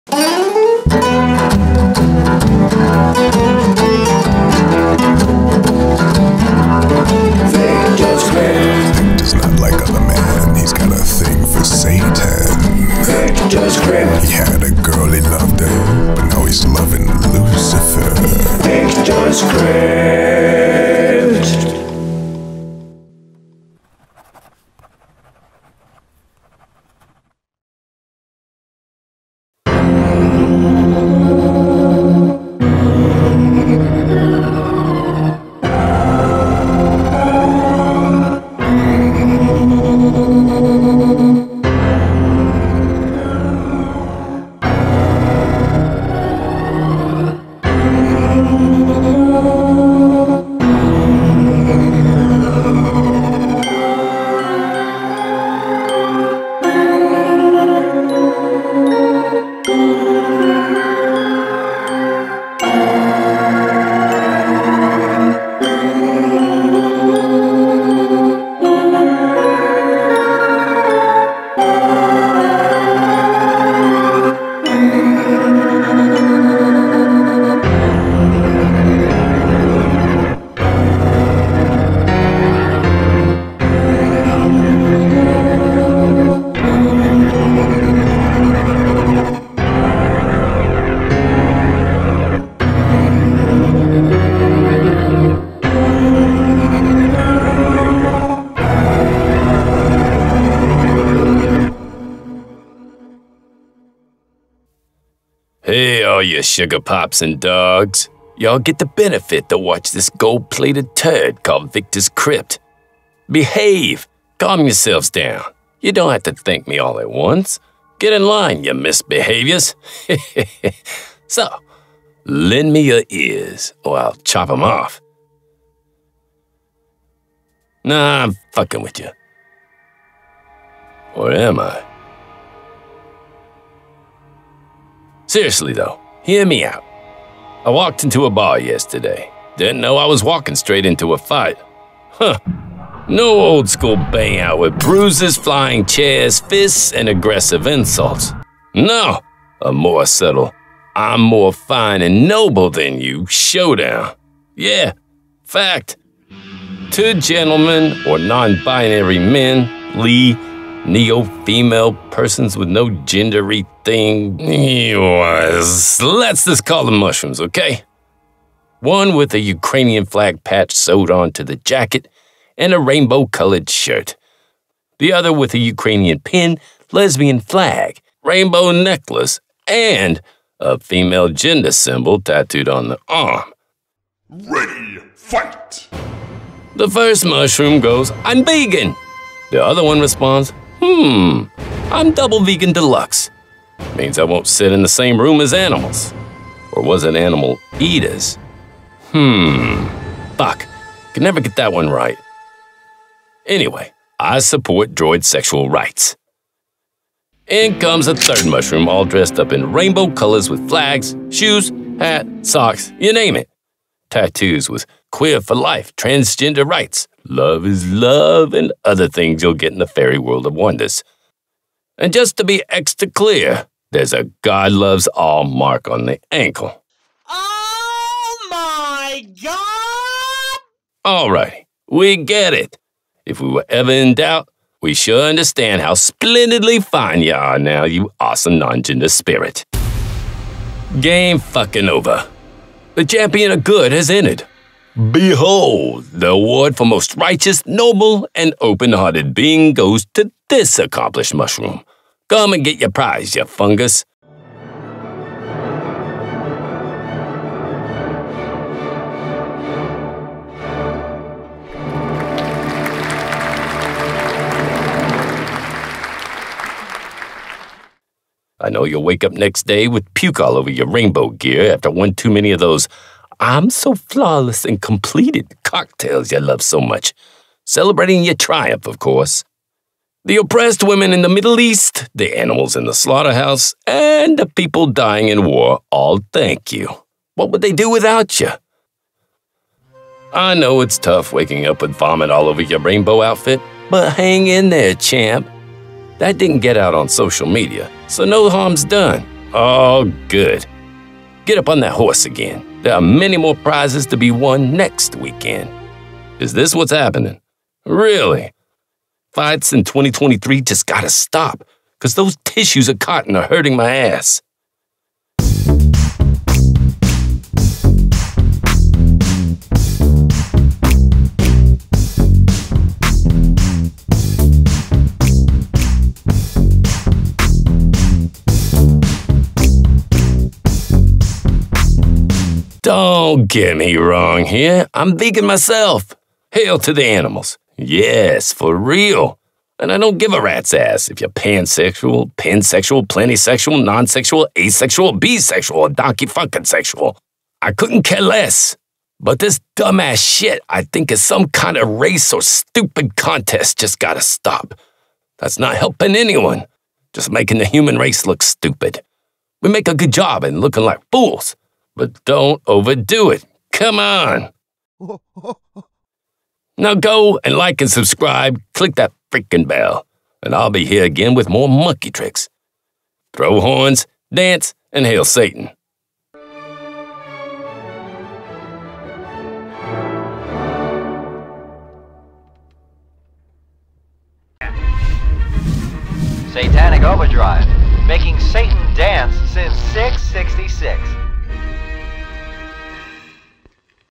Victors Crypt episode 54 - The Mushroom Showdown
There’s a new kind of showdown going on in our ”saloons” today.
God damn embarrassing for all mankind.
Fights in 2023 just gotta stop cause those cotton-tissues are hurting my ass!

Watch what Dr F*ck you over here is so worked up about in episode 54 of Victors Crypt ”The Mushroom Showdown”.

Throw horns, dance & hail Satan!

Music, script, filming, editing, acting: Victor Natas
Paintings & Art: Diana “Lucifera” Sundkvist, Cezilia Hjelt & Teres Kölbel

Be sure to like, follow and listen to Lucifers favorite orchestra Satanic Overdrive: